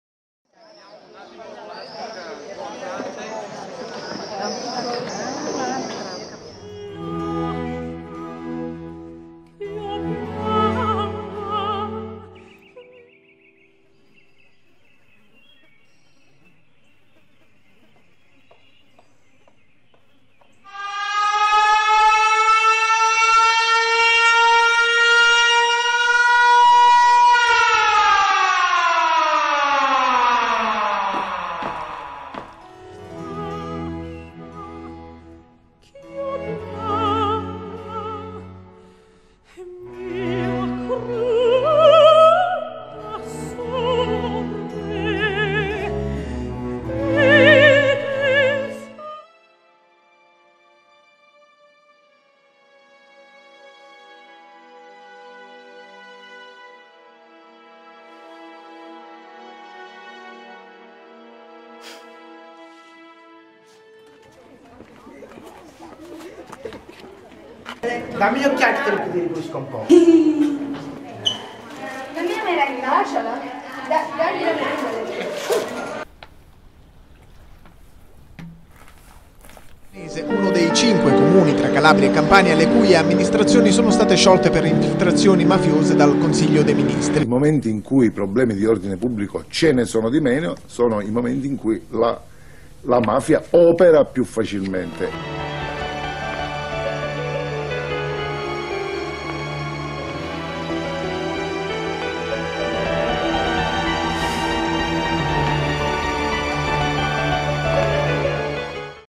MEMORIA IN CORTO Film Fest 2019 | Promo 14 maggio
14 maggio 2019 - 2a Edizione Memoria in Corto Film Fest.
Multisala Portanova, Crema (CR).
Filmmaker e cortometraggi da tutta Italia si contenderanno la Targa Memoria in Corto 2019.
Introduzione con il regista Luca Arcidiacono e l'attore Walter Nestola dove presenteranno il film 'Aggrappati a me'.